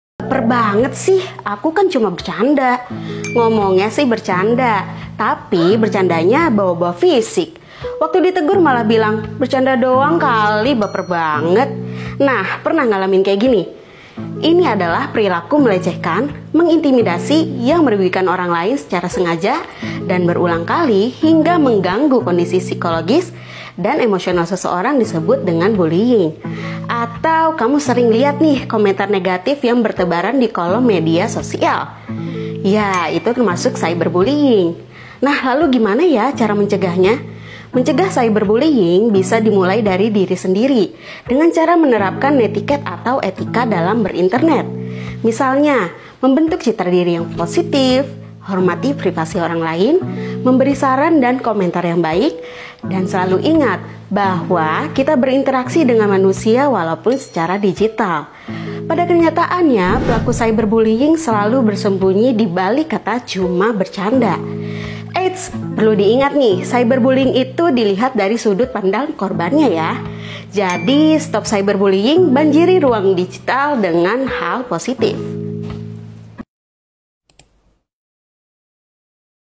Bullying dibalik kata "BERCANDA"
“Cuma bercanda doang, baper bgt lo!” Pernah ngalamin kaya gini? Simak deh videonya sampe akhir..